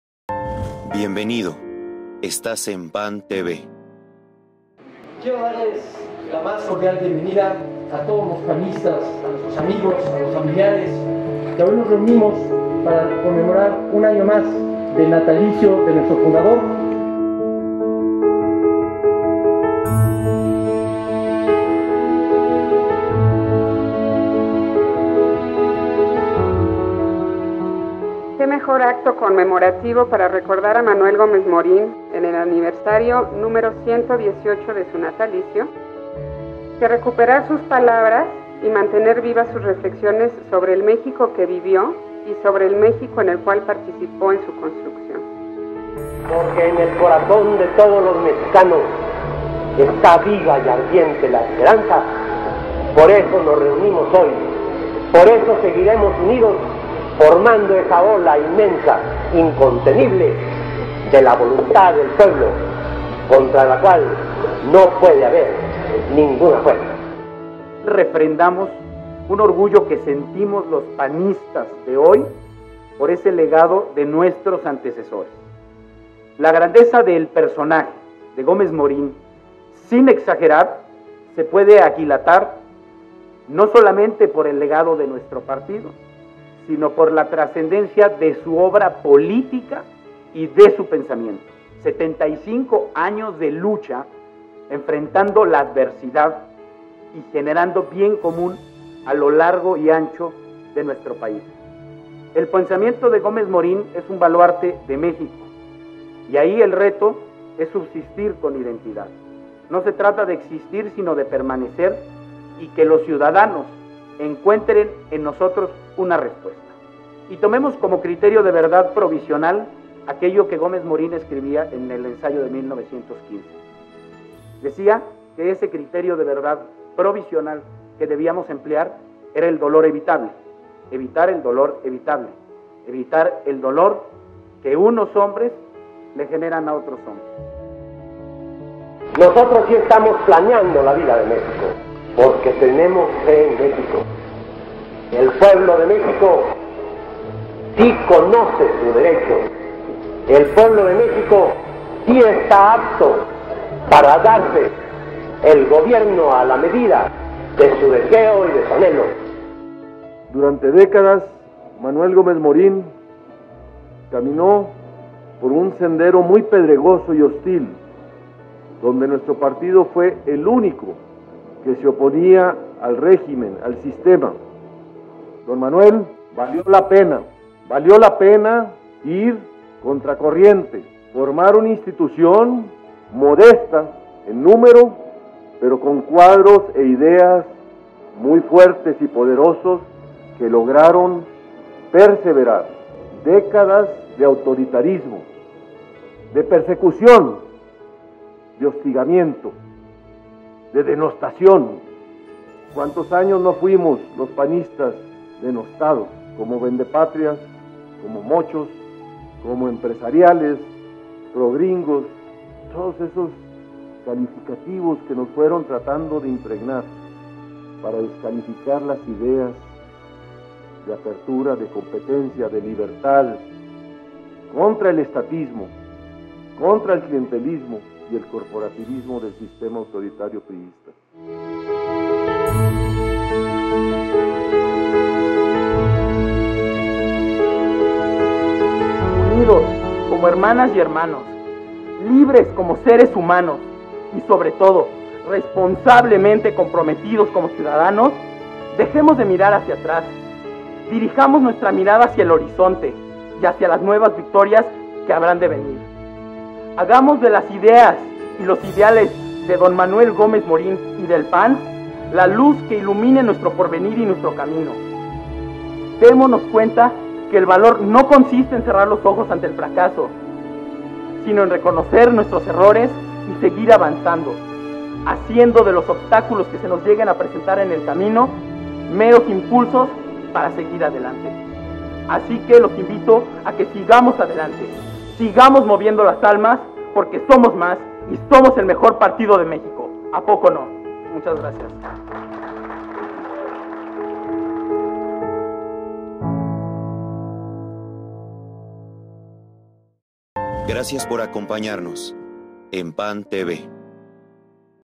118 Aniversario del Natalicio de Manuel Gómez Morin
Manuel Gómez Morin, nació en Batopilas, Chihuahua un 27 de febrero de 1897, fue creador de diversas instituciones en las que se resalta el Partido Acción Nacional en 1939, fue Rector de la Universidad Nacional Autónoma de México en 1933 y fue integrante del grupo denominado Los siete sabios de México de la Generación 1915.